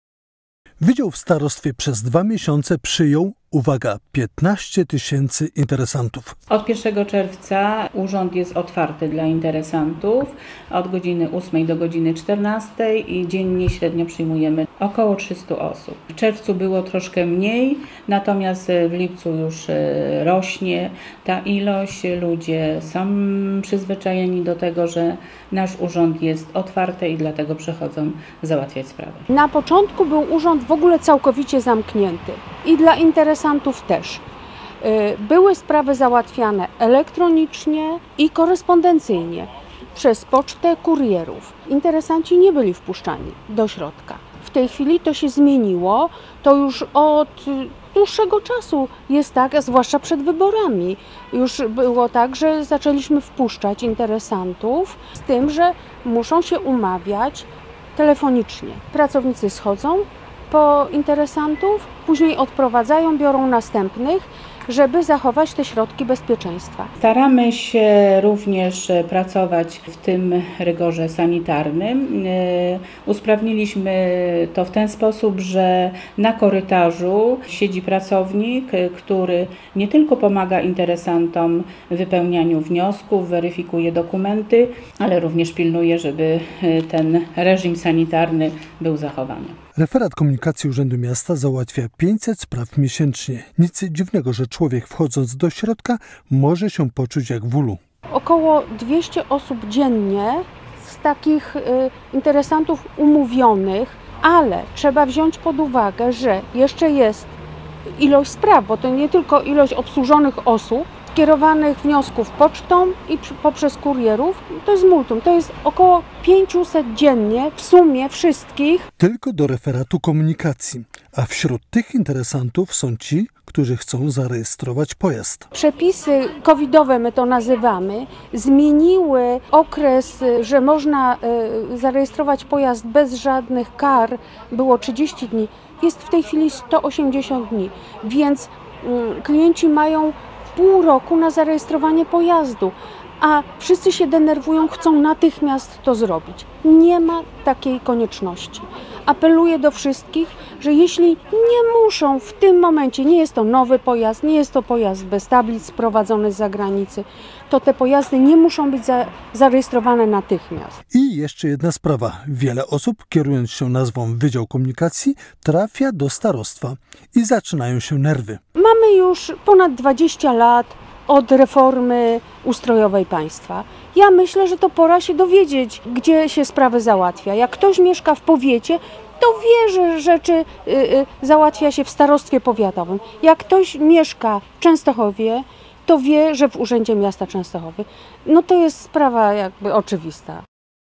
Wydziały komunikacji w czasie pandemii
W czasie pandemii wśród najbardziej narażonych na zakażenie są pracownicy wydziałów komunikacji zarówno w starostwie jak i w magistracie częstochowskim. Tam przewija się najwięcej interesantów. Dzisiaj przyglądamy się tym jednostkom.